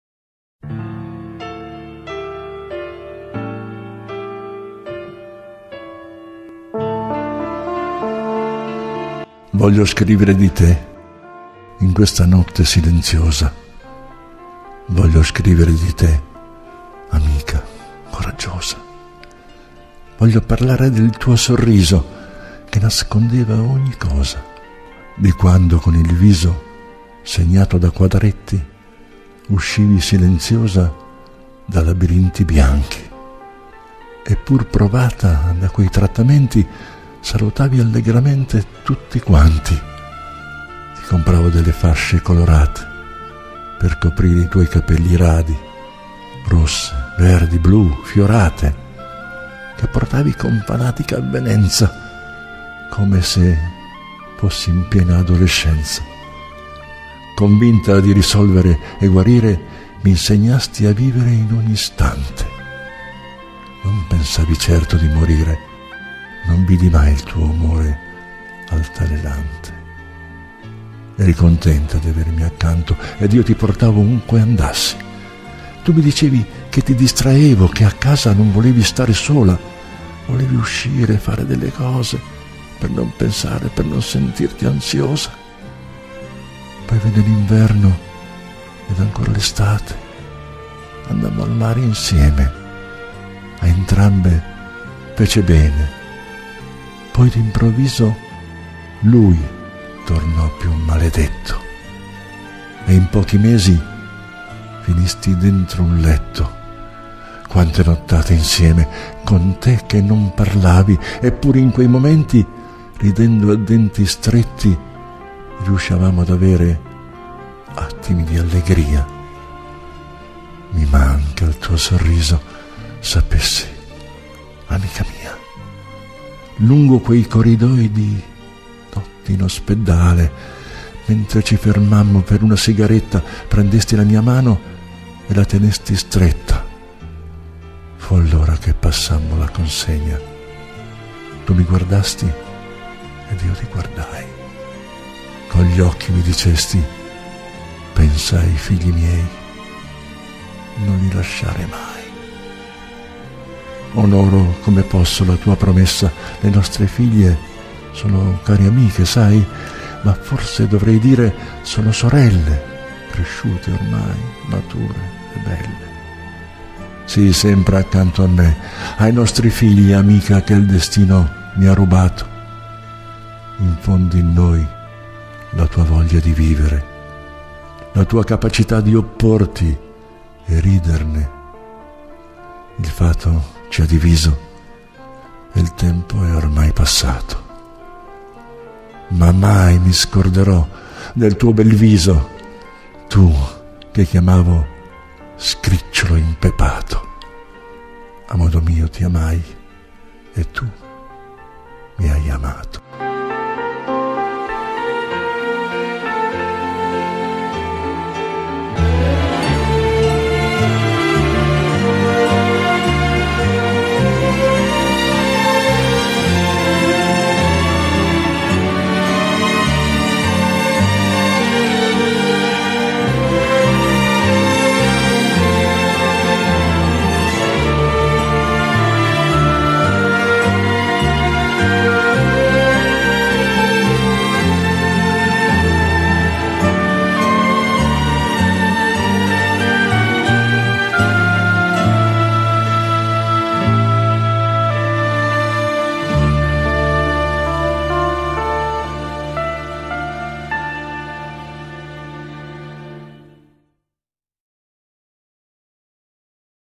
"Margherita" poesia di Emanuela Gentilini
"Margherita" di "Emanuela Gentilini"
Poesia classificatasi al 1° posto nella sezione “ TU  VIVI  DENTRO  E  FUORI  ME ” alla 3^ Edizione dell'Evento Letterario "SCRIVIAMO” 2015.
Montaggio video by "Phil Sine Die"